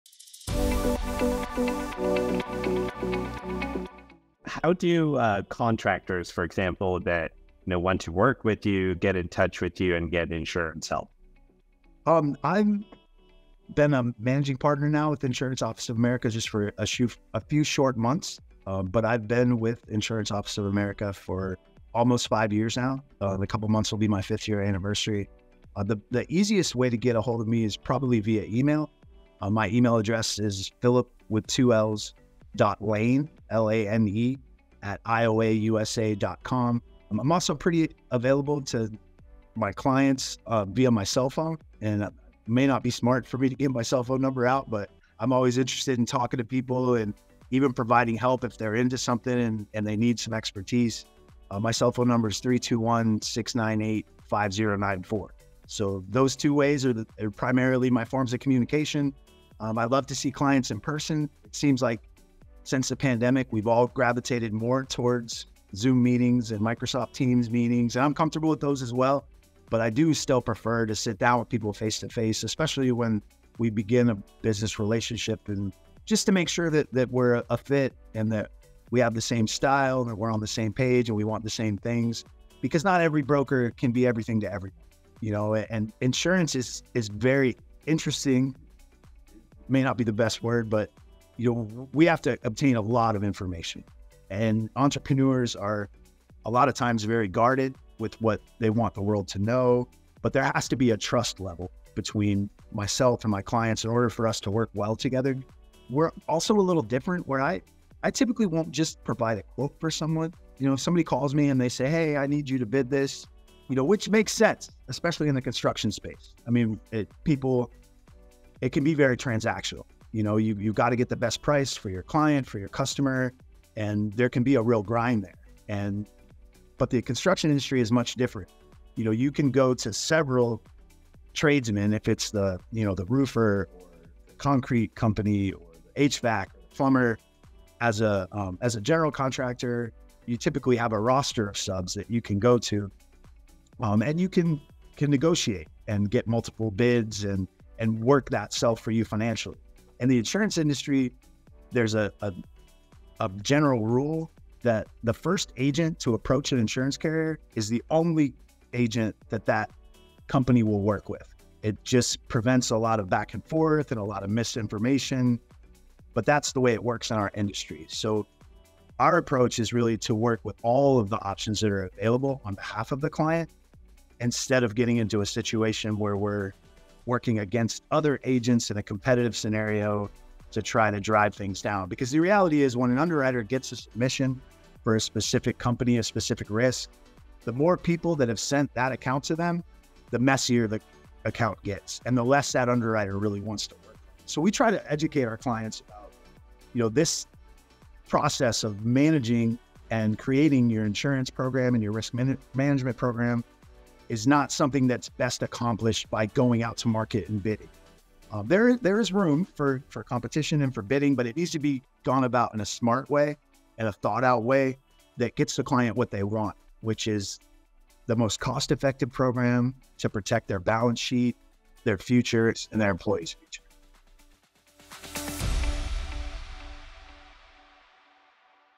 How do you get in touch with Phillip Lane?
Discover how to connect with Phillip Lane for any inquiries or discussions about construction insurance.